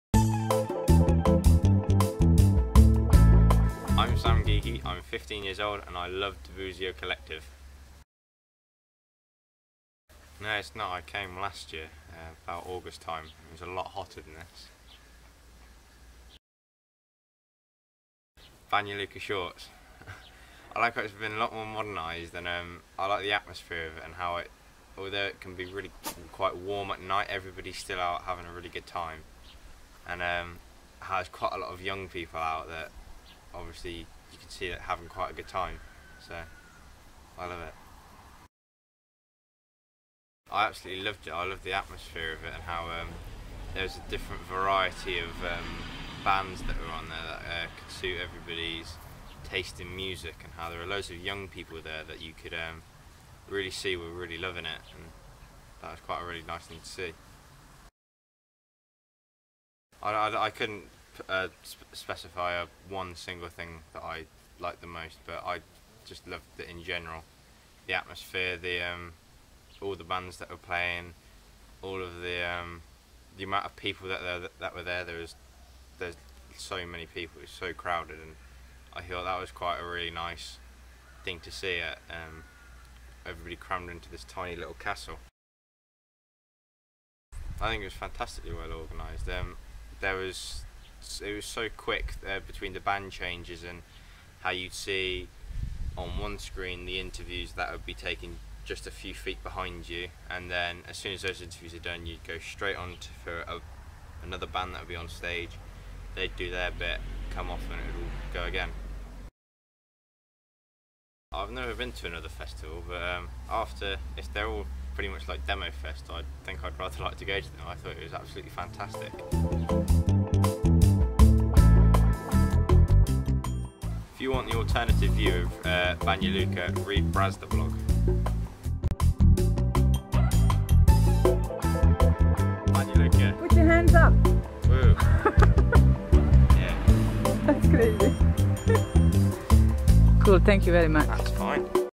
Brazda video - Interview with Sam Geekie by @cacawebdesign - Demofest Banja Luka
Demofest 2013 - Impressions of Banja Luka from a Visitor